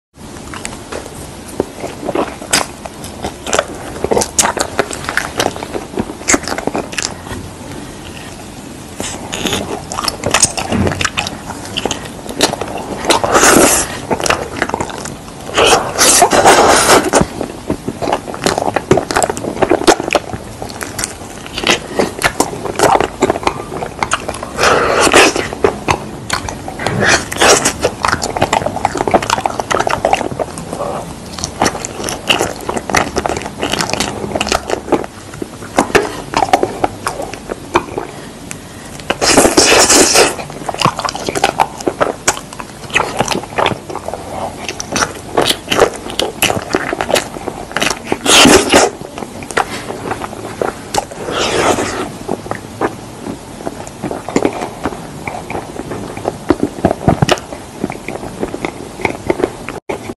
🍬What's the SECRET to this IRRESISTIBLE Food?🍬
Join me for the ultimate food adventure in this mukbang video! From delicious mochi to mouthwatering treats, you won't want to miss out on the best food ever! Sit back, relax, and enjoy some ASMR eating sounds in this satisfying video.

Prepare yourself for a gastronomic adventure as this mukbang sensation takes you on an unforgettable journey through the most extraordinary and unconventional dishes you could ever imagine. From exotic delicacies to mind-bending flavor combinations, this video will leave you questioning everything you thought you knew about food.

Join our fearless host as they fearlessly indulge in a feast fit for the bravest of food enthusiasts. With each bite, they unlock a world of tastes, textures, and aromas that defy expectations and redefine what it means to truly savor a meal.